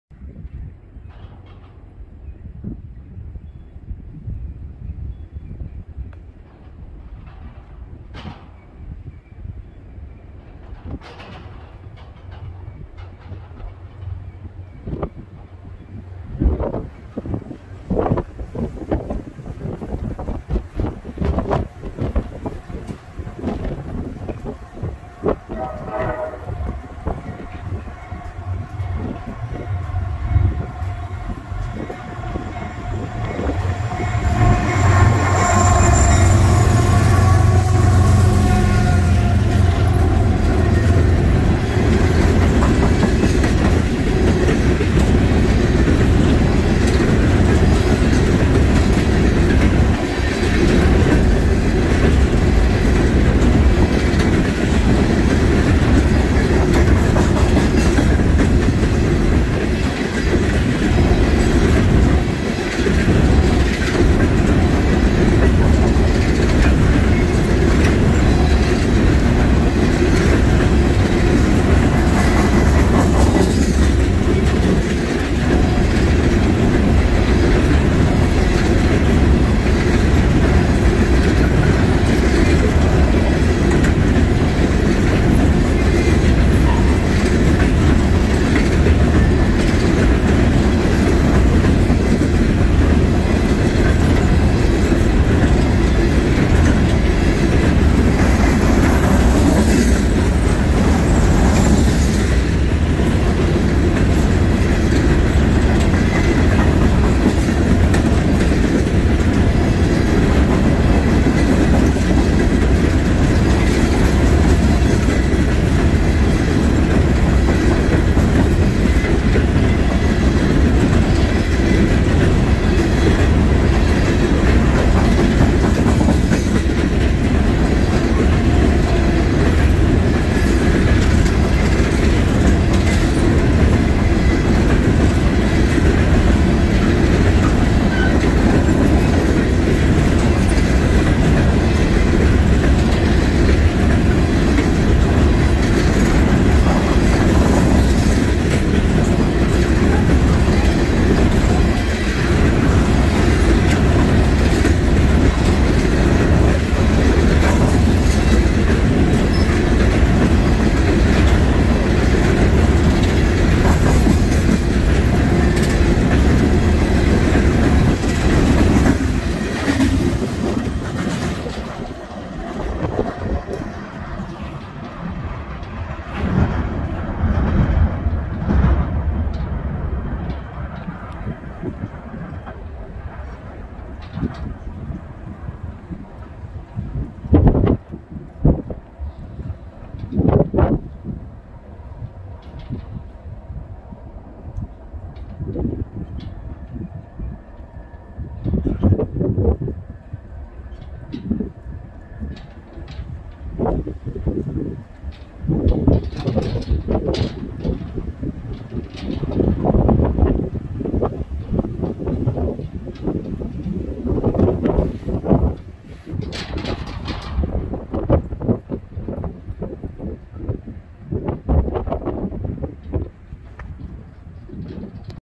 Norfolk Southern 4486 leads an empty coal train at Lewistown, PA (02/27/2022)
Crossing from track 1 to track 2 ;